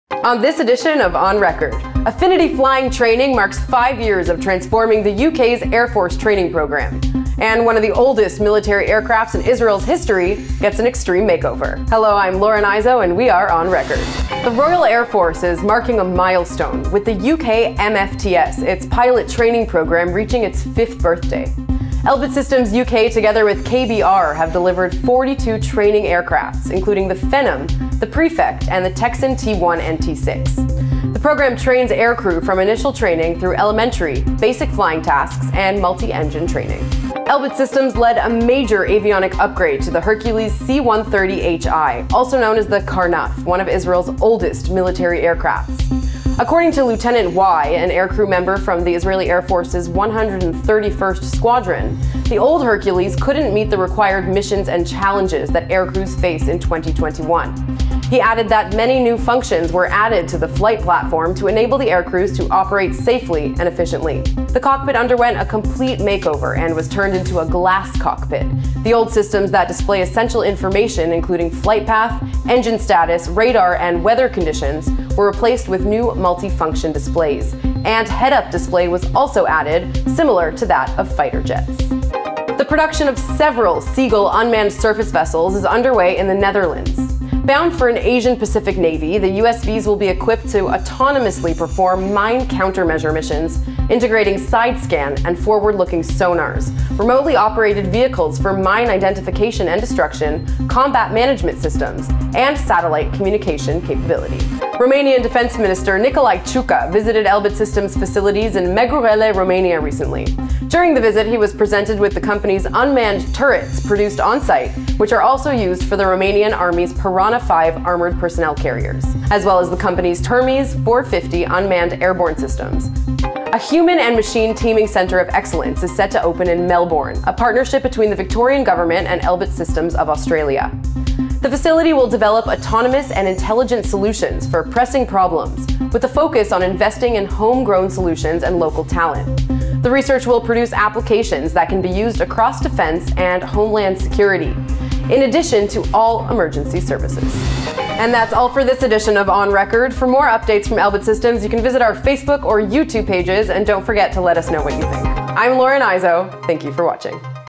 ON RECORD February 2021, second edition
This time OnRecord
00:15 Affinity Flying Training Services celebrates five years of training UK future pilots.
00:37 Avionics Upgrade to the Hercules C-130HI
01:22 Commencing production of Seagull USV for an Asia-Pacific customer.
01:45 Romanian Minister of Defense visits our facilities in Măgurele, Romania
02:03 Elbit Systems of Australia teams up with the Victorian government on AI research centre.